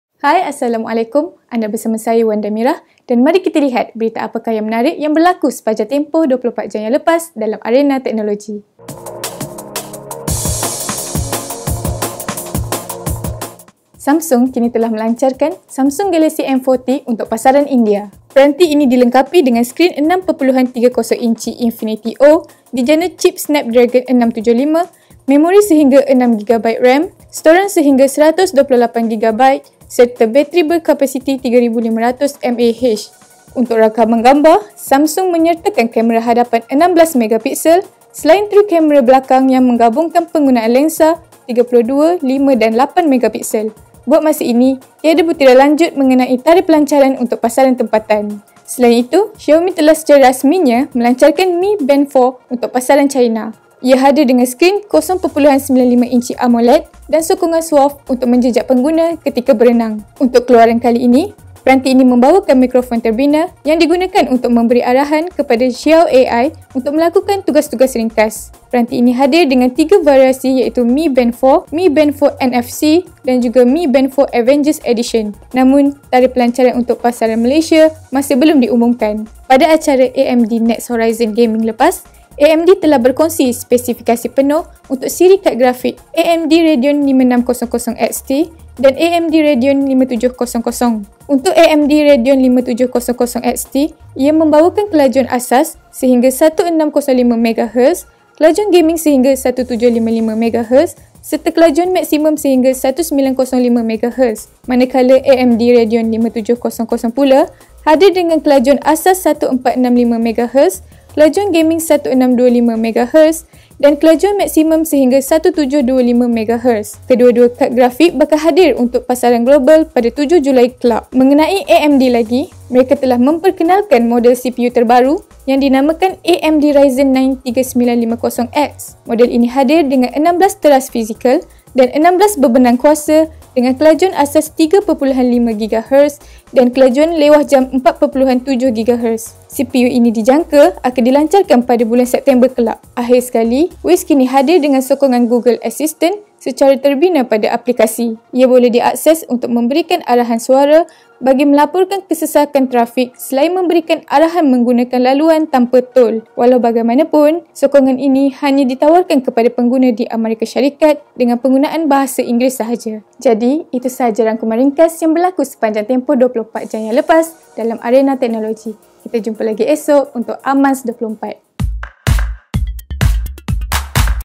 Amanz24 - Samsung Galaxy M40, Mi Band 4 Avengers, AMD Ryzen Baru
Samsung kini telah melancarkan Samsung Galaxy M40 untuk pasaran India. Peranti ini dilengkapi dengan skrin 6.30-inci Infinity-O, dijana cip Snapdragon 675, memori sehingga 6GB RAM, storan sehingga 128GB serta bateri berkapasiti 3500mAh. Untuk rakaman gambar, Samsung menyertakan kamera hadapan 16-megapixel, selain tri-kamera belakang yang mengabungkan penggunaan lensa 32+ 5+ 8-megapixel. Buat masa ini, tiada butiran lanjut mengenai tarikh pelancaran untuk pasaran tempatan.

Selain itu, Xiaomi telah secara rasminya melancarkan Mi Band 4 untuk pasaran China. Ia hadir dengan skrin 0.95-inci AMOLED dan sokongan SWOLF untuk menjejak pengguna ketika berenang. Untuk keluaran kali ini, peranti ini membawakan mikrofon terbina yang digunakan untuk memberi arahan kepada Xiao AI untuk melakukan tugas-tugas ringkas. Peranti ini hadir dengan tiga variasi iaitu Mi Band 4, Mi Band 4 NFC dan juga Mi Band 4 Avengers Edition. Namun, tarikh pelancaran untuk pasaran Malaysia masih belum diumumkan.

Pada acara AMD Next Horizon Gaming lepas, AMD telah berkongsi spesifikasi penuh untuk siri kad grafik, AMD Radeon 5600 XT dan AMD Radeon 5700. Untuk AMD Radeon 5700 XT, ia membawakan kelajuan asas sehingga 1605MHz, kelajuan gaming sehingga 1755MHz serta kelajuan maksimum sehingga 1905MHz. Manakala AMD Radeon 5700 pula hadir dengan kelajuan asas 1465MHz, kelajuan gaming 1625MHz, dan kelajuan maksimum sehingga 1725MHz. Kedua-dua kad grafik bakal hadir untuk pasaran global pada 7 Julai kelak.

Mengenai AMD lagi, mereka telah memperkenalkan model CPU terbaru yang dinamakan AMD Ryzen 9 3950X. Model ini hadir dengan 16 teras fizikal dan 16 bebenang kuasa dengan kelajuan asas 3.5GHz dan kelajuan lewahjam 4.7GHz. CPU ini dijangka akan dilancarkan pada bulan September kelak.

Akhir sekali, Waze kini hadir dengan sokongan Google Assistant secara terbina pada aplikasi. Ia boleh diakses untuk memberikan arahan suara bagi melaporkan kesesakan trafik selain memberikan arahan menggunakan laluan tanpa tol. Walau bagaimanapun, sokongan ini hanya ditawarkan kepada pengguna di Amerika Syarikat dengan penggunaan bahasa Inggeris sahaja.